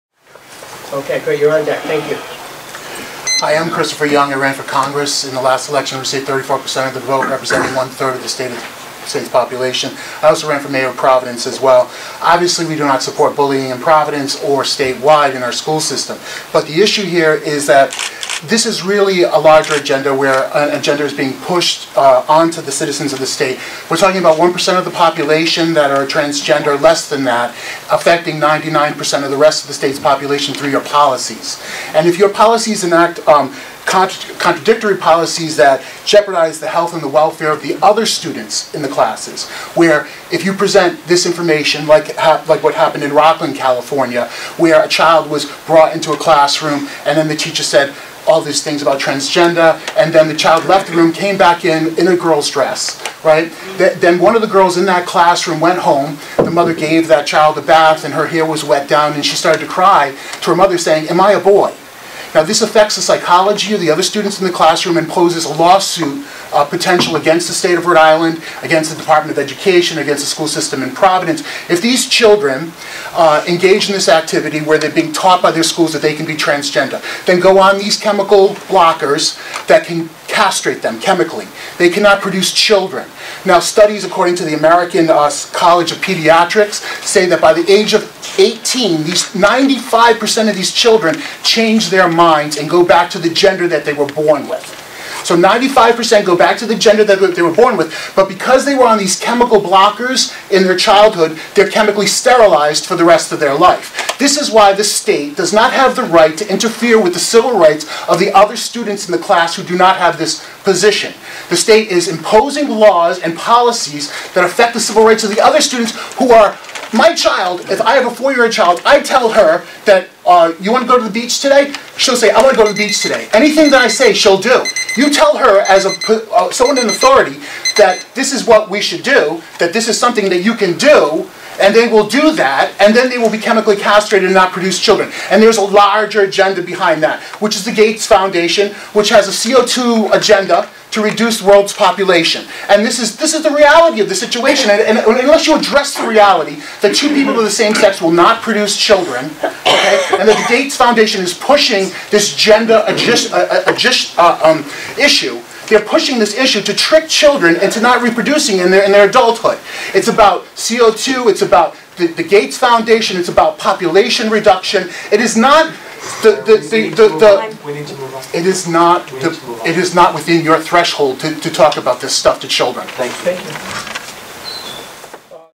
Transgender Policy Debate: Protecting Students or Pushing an Agenda?
Christopher Young, a former congressional and Providence mayoral candidate, provides testimony during a Rhode Island Department of Education (RIDE) hearing on February 7, 2018, regarding transgender student policies. Speaking in Providence, Young expresses concerns about the implementation of policies affecting transgender students, emphasizing potential psychological impacts on other children in classrooms. He references an incident in Rocklin, California, where a classroom discussion about transgender identity reportedly led to confusion for a student. Young raises questions about the use of chemical blockers, citing the American College of Pediatrics, and argues that such policies could infringe on the civil rights of non-transgender students. He also suggests a broader agenda tied to population reduction, mentioning the Gates Foundation and its alleged goals. The testimony highlights ongoing debates surrounding education policy, student rights, and societal implications.